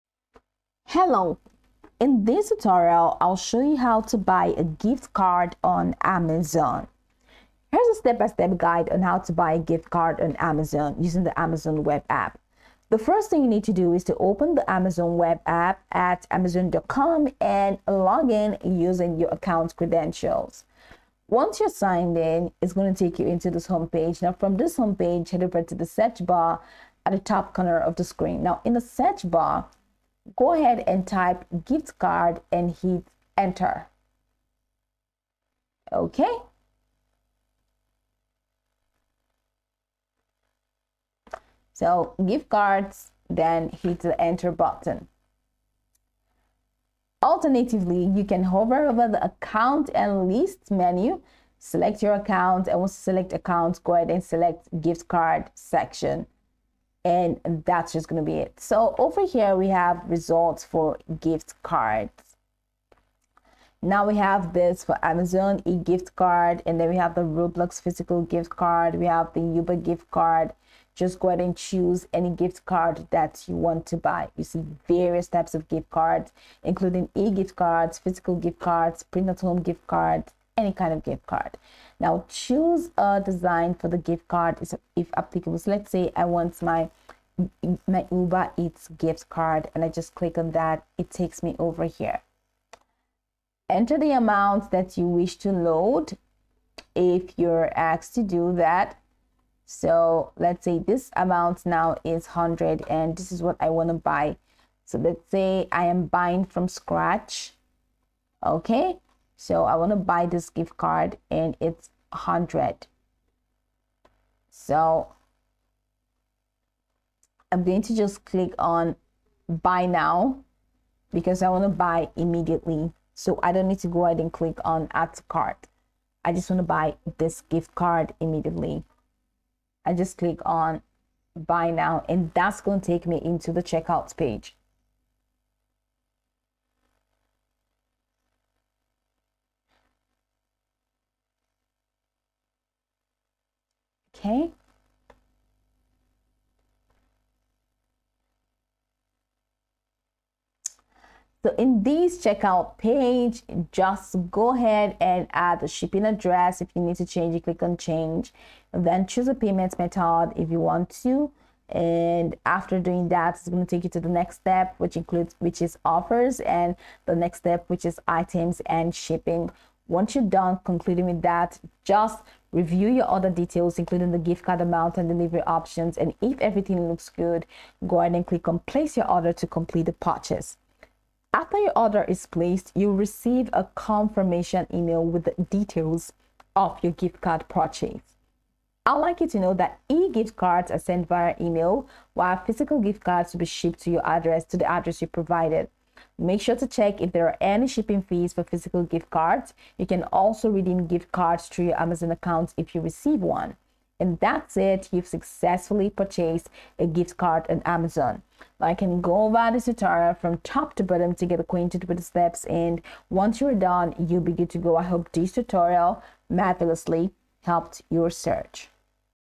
How To Buy A Gift Card On Amazon (Quick Tutorial)
In this video I will show you How To Buy A Gift Card On Amazon. It would be good if you watch the video until the end so that you don't miss important steps.